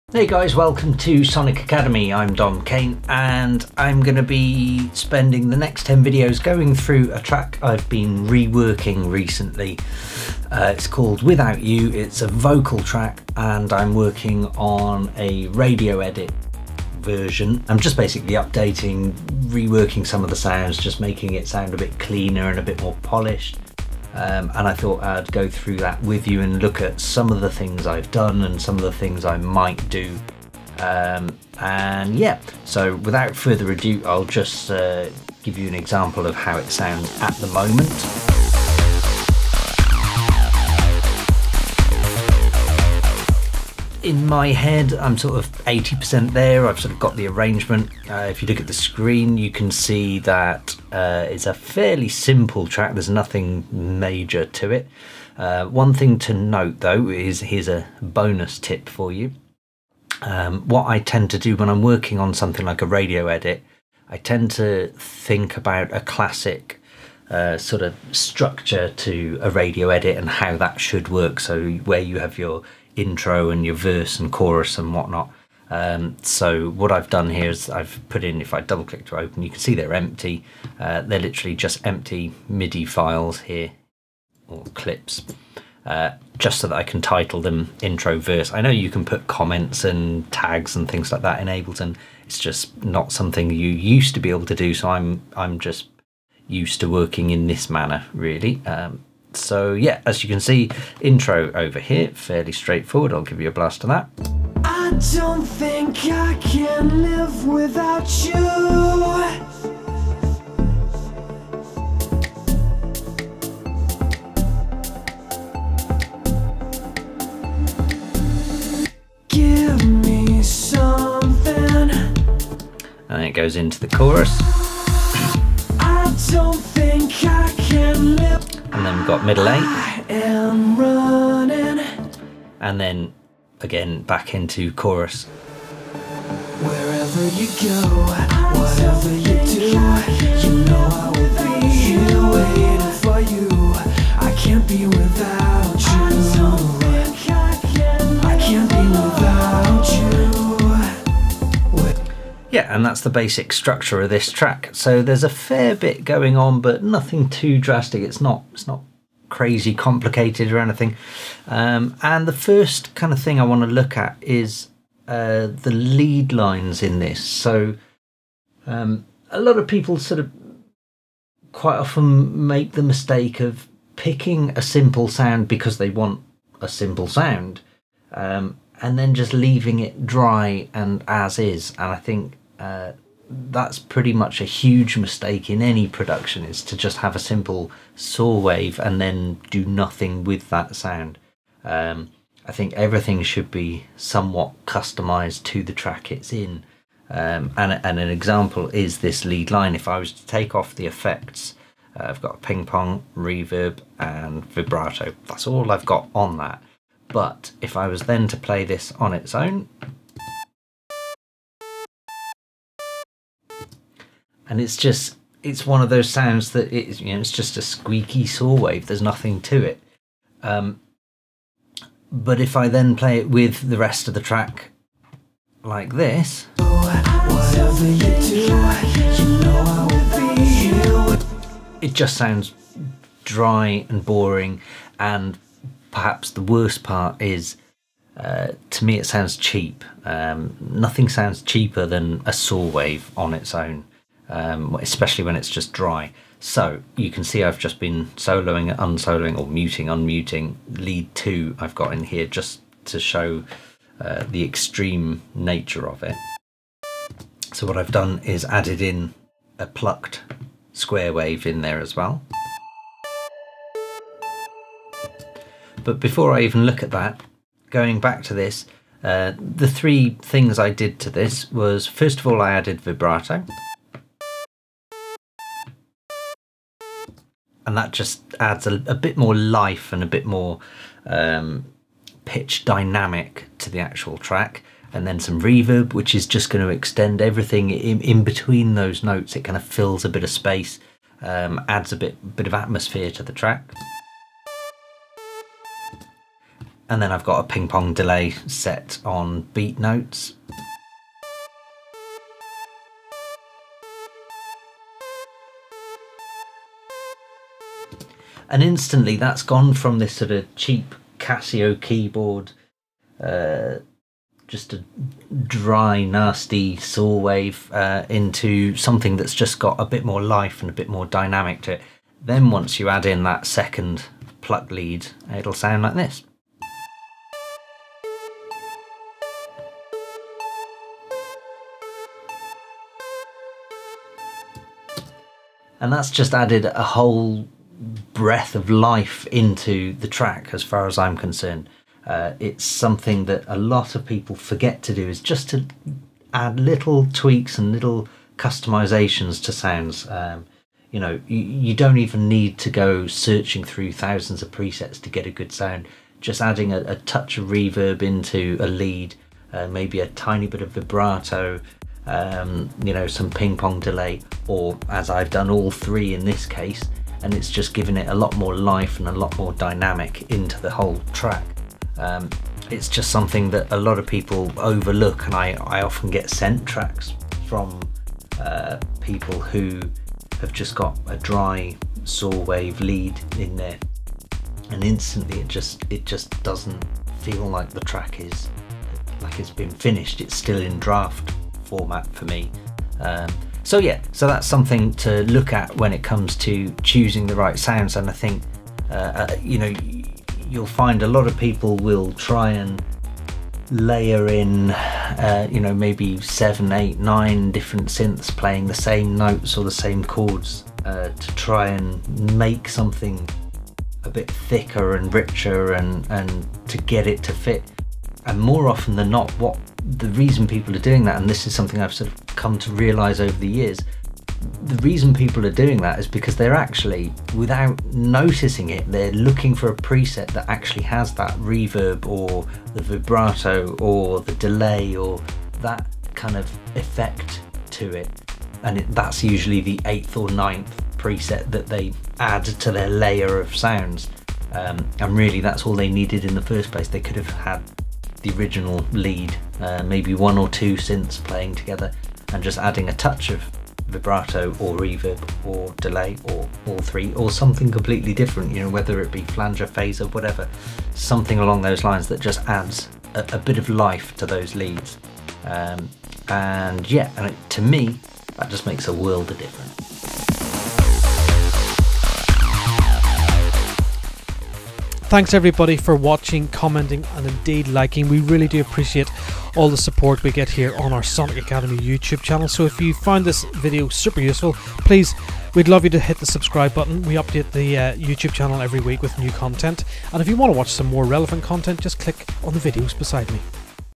Adding Life to Leads in Ableton Live with Dom Kane - Tech Tip
Mau5trap's Dom Kane is back with us once again this week for a new set of Tech Tips, this time in Ableton as we join him in his studio where he's working on a radio edit.

Over these 10 videos we get a detailed insight into some of his pro tips and tricks he uses to fine tune, speed up workflow, enhance and ultimately finalise a track.

These tips are all about keeping things simple and taking time to choose the correct sounds and methods to get from A to B so you're not re-visiting elements over and over.
Covering aspects such as where to place your compressor in the chain, or if you even need one at all, to adding stereo or presence to different elements and EQing certain frequencies to keep your tracks clean and make them stand out.

A set of videos that reminds us that taking care in your choices can be super beneficial in your overall satisfaction when reaching the final stages of your production.